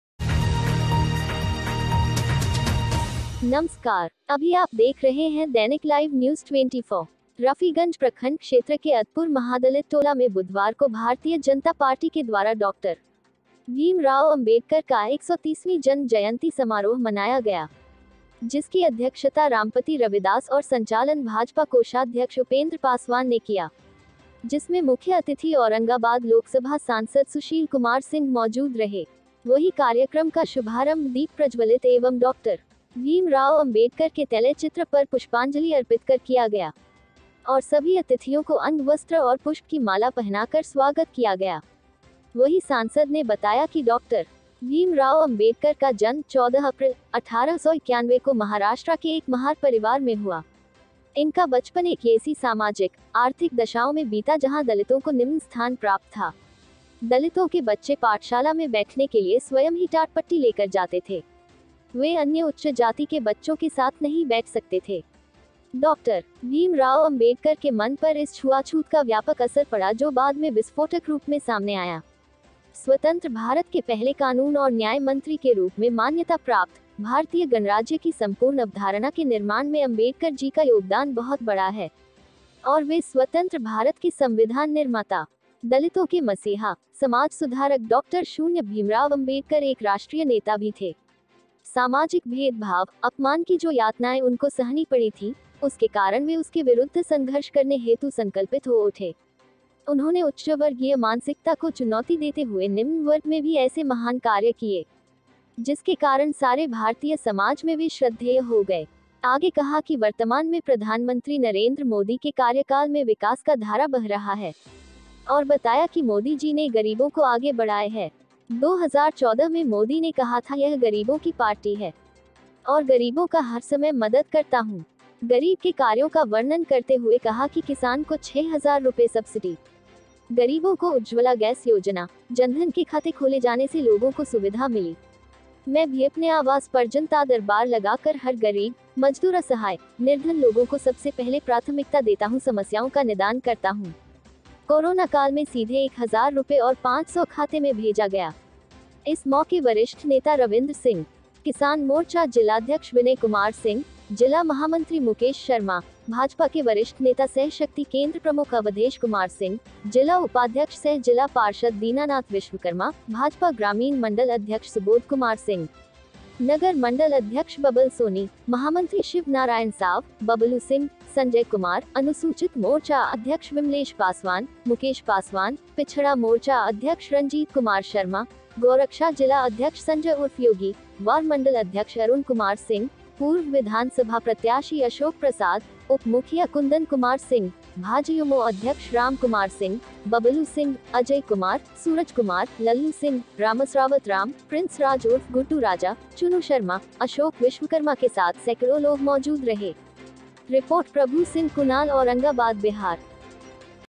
डॉ.भीम राव अंबेडकर का 130वीं जन्मदिवस मनाया गया,सांसद सुशील कुमार सिंह मौजूद रहे।
कुणाल सिंह औरंगाबाद

रफीगंज प्रखंड क्षेत्र के अदलपुर महादलित टोला में बुधवार को भारतीय जनता पार्टी के द्वारा डॉ.भीम राव अंबेडकर का 130वीं जन्म जयंती समारोह मनाया गया।जिसकी अध्यक्षता रामपति रविदास और संचालन भाजपा कोषाध्यक्ष उपेंद्र पासवान ने किया।जिसमें मुख्य अतिथि औरंगाबाद लोकसभा सांसद सुशील कुमार सिंह मौजूद रहे।वही कार्यक्रम का शुभारंभ दीप प्रज्वलित एवं डॉ.भीम राव अंबेडकर के तैलय चित्र पर पुष्पांजलि अर्पित कर किया गया।और सभी अतिथियों को अंग वस्त्र और पुष्प की माला पहनाकर स्वागत किया गया।वही सांसद ने बताया कि डॉ. भीम राव अंबेडकर का जन्म 14 अप्रैल,1891 को महाराष्ट्र के एक महार परिवार में हुआ। इनका बचपन एक येसी सामाजिक,आर्थिक दशाओं में बीता जहां दलितों को निम्न स्थान प्राप्त था। दलितों के बच्चे पाठशाला में बैठने के लिए स्वयं ही टाट-पट्टी लेकर जाते थे। वे अन्य उच्च जाति के बच्चों के साथ नहीं बैठ सकते थे।डॉ. भीम राव अम्बेडकर के मन पर इस छुआछूत का व्यापक असर पड़ा जो बाद में विस्फोटक रूप में सामने आया। स्वतंत्र भारत के पहले कानून और न्याय मंत्री के रूप में मान्यता प्राप्त,भारतीय गणराज्य की संपूर्ण अवधारणा के निर्माण में अम्बेडकर जी का योगदान बहुत बड़ा है।और वे स्वतन्त्र भारत के संविधान निर्माता,दलितों के मसीहा,समाज सुधारक डॉ० भीमराव अम्बेडकर एक राष्ट्रीय नेता भी थे। सामाजिक भेदभाव,अपमान की जो यातनाएं उनको सहनी पड़ी थीं,उसके कारण वे उसके विरुद्ध संघर्ष करने हेतु संकल्पित हो उठे।उन्होंने उच्चवर्गीय मानसिकता को चुनौती देते हुए निम्न वर्ग में भी ऐसे महान कार्य किये,जिसके कारण सारे भारतीय समाज में वे श्रद्धेय हो गये।आगे कहा कि वर्तमान में प्रधानमंत्री नरेन्द्र मोदी के कार्यकाल में विकास का धारा बह रहा है। और बताया कि मोदी जी ने गरीबों को आगे बढ़ाये है।2014 में मोदी ने कहा था यह गरीबों की पार्टी है। और गरीबों का हरसमय मदद करता हूं। गरीब के कार्यों का वर्णन करते हुए कहा कि किसान को ₹6000 सब्सिडी ,गरीबों को उज्ज्वला गैस योजना,जनधन के खाते खोले जाने से लोगों को सुविधा मिली। मैं भीअपनेआवास परजनतादरबार लगाकर हर गरीब, मजदुर असहाय, निर्धन लोगों को सबसे पहले प्राथमिकता देता हूं समस्याओं का निदान करता हूं।कोरोना काल में सीधे ₹ 1000 और 500 खाते में भेजा गया।इस मौके वरिष्ठ नेता रविन्द्र सिंह,किसान मोर्चा जिलाध्यक्ष विनय कुमार सिंह,जिला महामंत्री मुकेश शर्मा, भाजपा के वरिष्ठ नेता सह शक्ति केंद्र प्रमुख अवधेश कुमार सिंह,जिला उपाध्यक्ष सह जिला पार्षद दीनानाथ विश्वकर्मा, भाजपा ग्रामीण मंडल अध्यक्ष सुबोध कुमार सिंह,नगर मंडल अध्यक्ष बबल सोनी,महामंत्री शिव नारायण साव, बबलु सिंह,संजय कुमार,अनुसूचित मोर्चा अध्यक्ष विमलेश पासवान,मुकेश पासवान,पिछड़ा मोर्चा अध्यक्ष रंजीत कुमार शर्मा,गौ रक्षा जिला अध्यक्ष संजय उर्फ योगी,वार मंडल अध्यक्ष अरुण कुमार सिंह,पूर्व विधानसभा प्रत्याशी अशोक प्रसाद,उप मुखिया कुंदन कुमार सिंह,भाजयुमो अध्यक्ष राम कुमार सिंह,बबलु सिंह, अजय कुमार,सूरज कुमार,लल्लू सिंह,रामसरावत राम,प्रिंस राज उर्फ गुड्डू राजा,चुनु शर्मा,अशोक विश्वकर्मा के साथ सैकड़ो लोग मौजूद रहे।